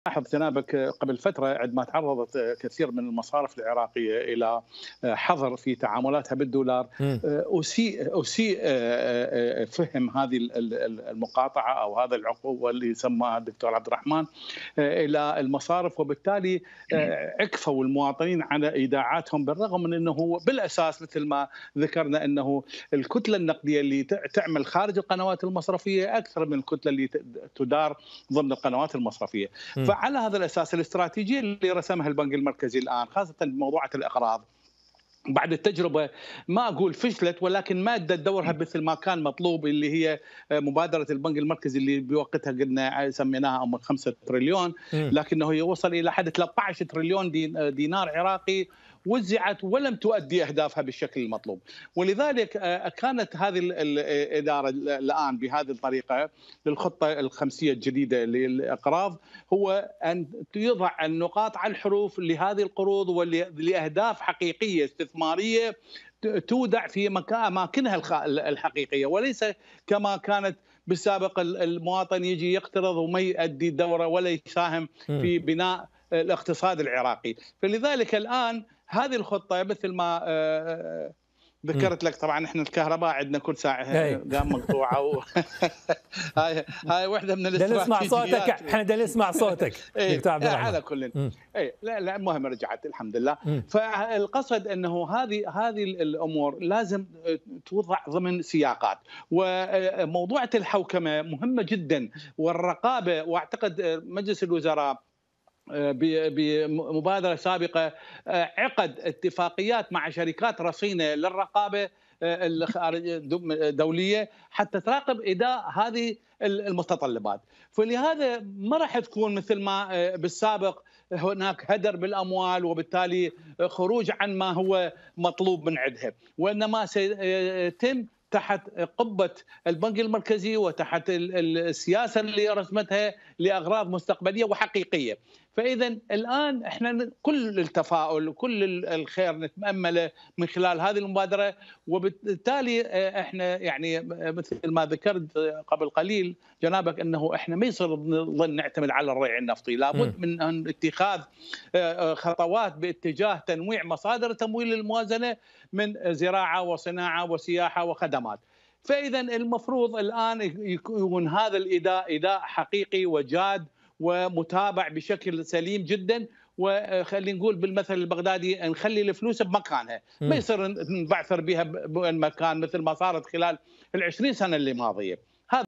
سعي حكومي لرفع مستوى التنمية الاقتصادية
خبير مالي ومصرفي يوضح أسباب الإساءة الى سمعة البنك العراقي وهذه الطريقة التي يتم استعادة الثقة بالنظام المصرفي

حمل تطبيق "الحرة كونيكت" للتلفزيونات والهواتف الذكية:

أمازون فاير